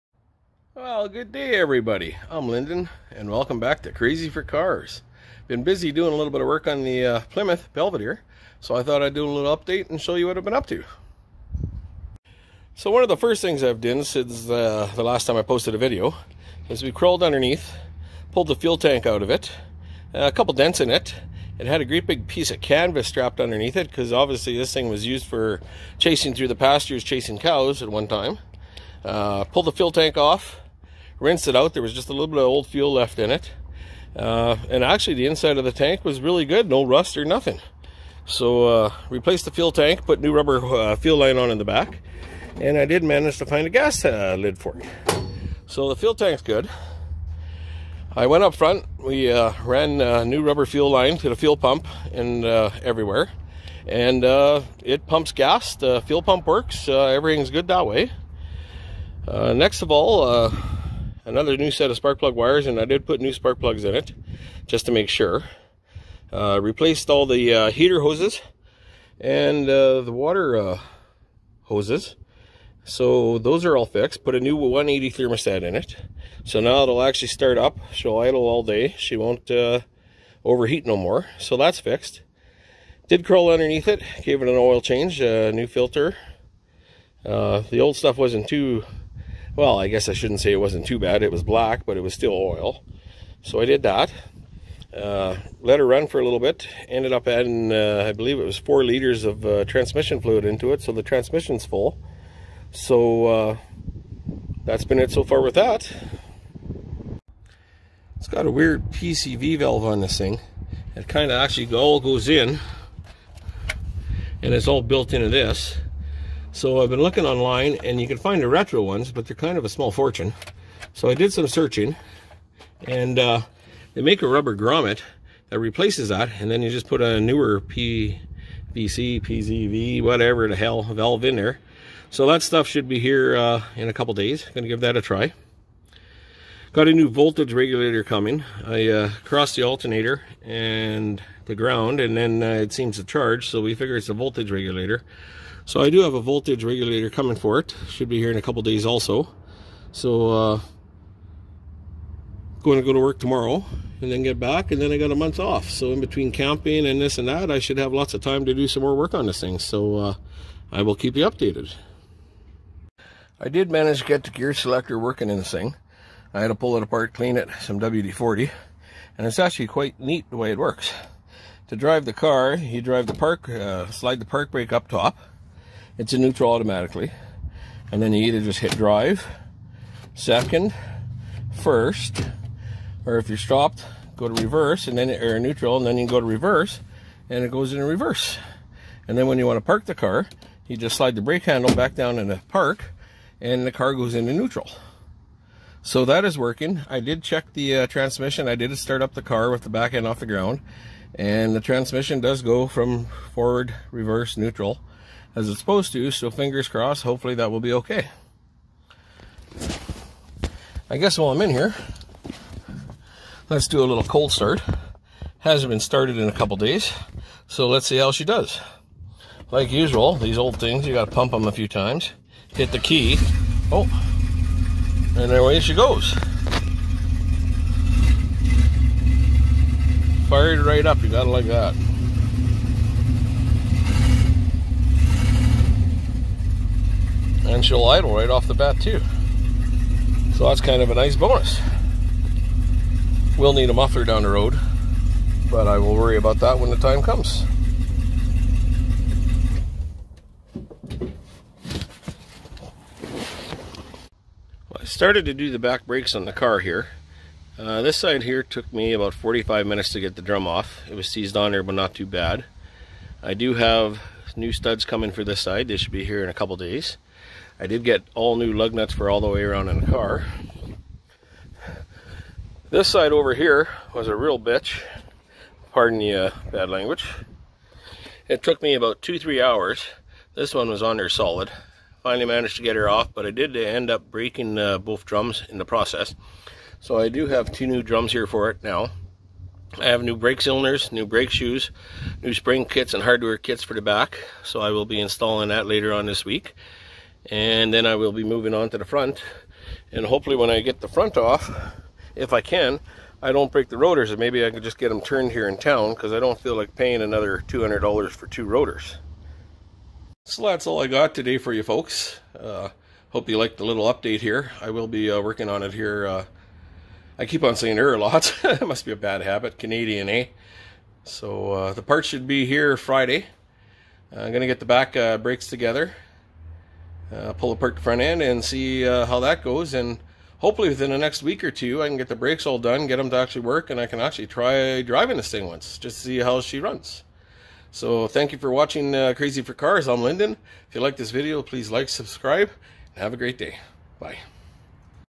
Dad's Car Part 4
Just a little update on the progress of getting my dads old 64 Belvedere back on the road.